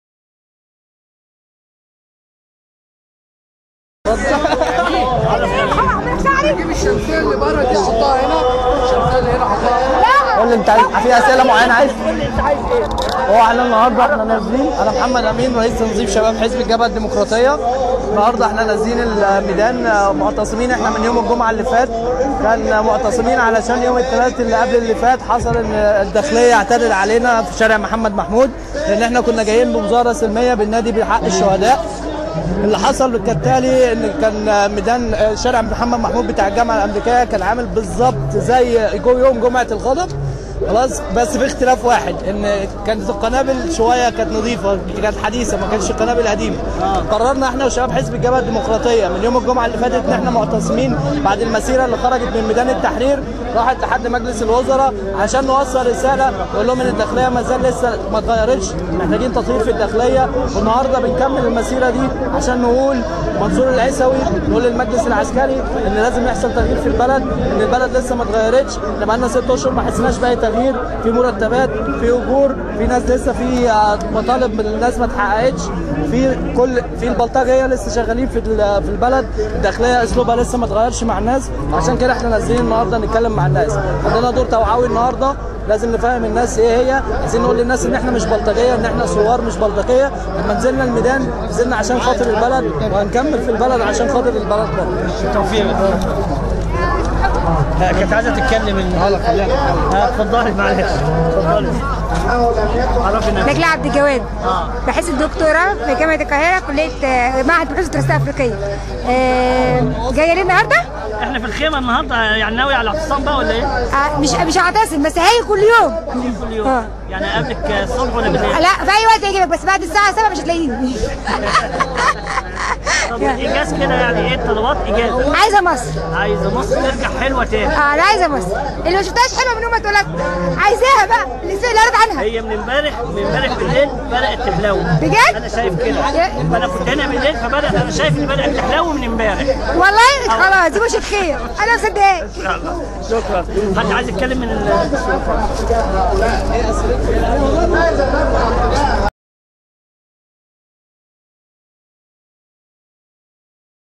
من داخل خيمة حزب الجبهة الديموقراطية فى جمعة 8 يوليو
من وسط ميدان التحرير من داخل خيمة حزب الجبهة الديموقراطية فى جمعة 8 يوليو